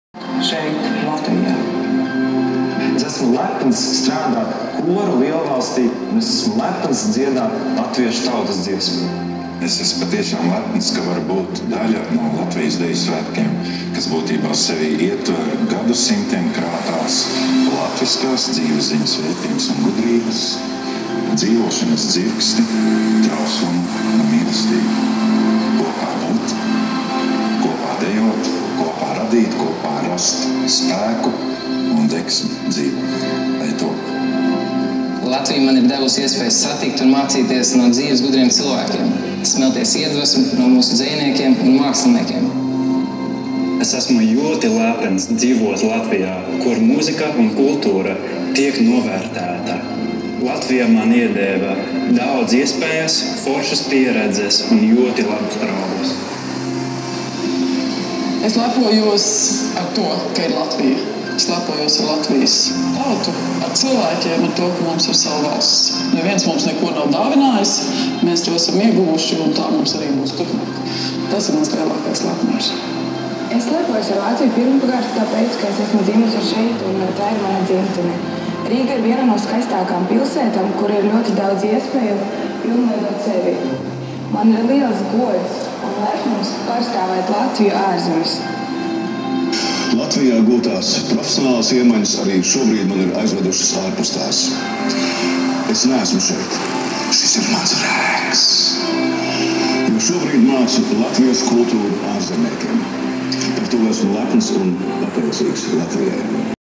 Happy Birthday Latvia !
On this day, the Republic of Latvia celebrates 99 years since it was proclaimed as a republic on November 18, 1918.